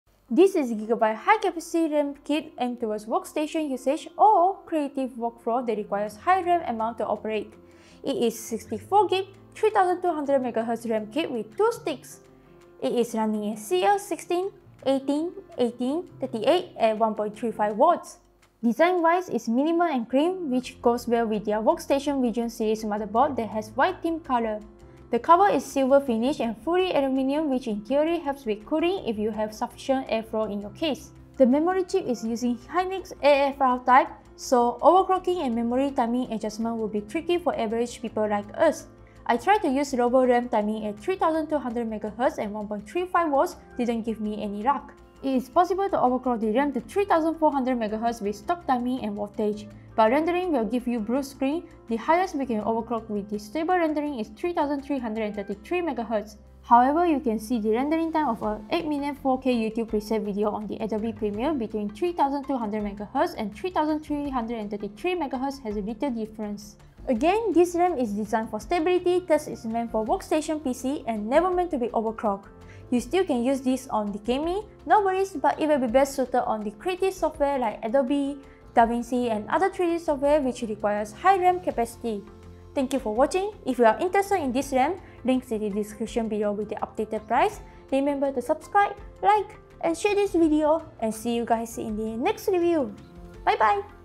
GIGABYTE DESIGNARE Memory Review | 64GB 3200MHz Workstation RAM Kit
Look for a workstation-grade RAM with high 64GB capacity to run that Video Editing or Multi-Layer Photoshop smoothly? Consider this RAM here :)

Video host: Youmi (Japanese)
Follow her socials: